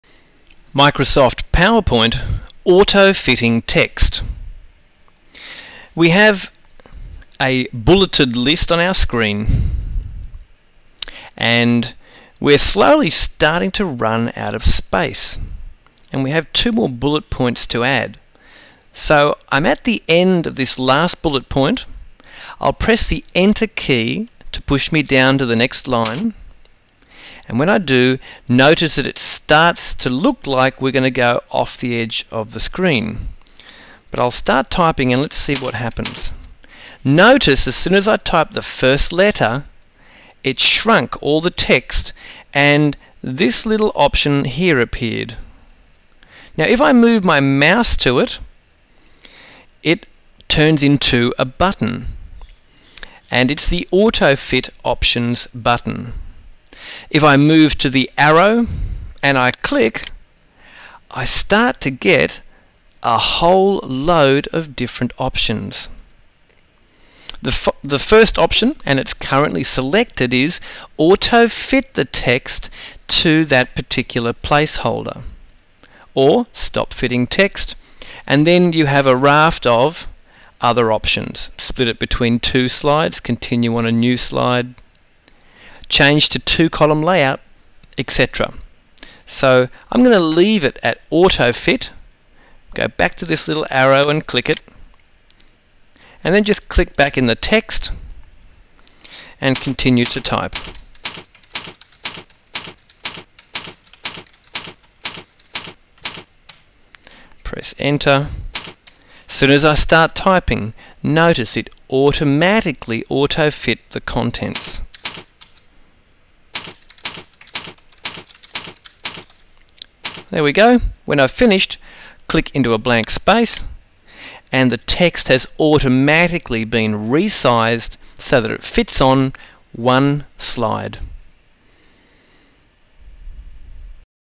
401404 PowerPoint Training Course - AutoFit Text Options
Microsoft PowerPoint will automatically resize text to fit within the space you give (if you want it to).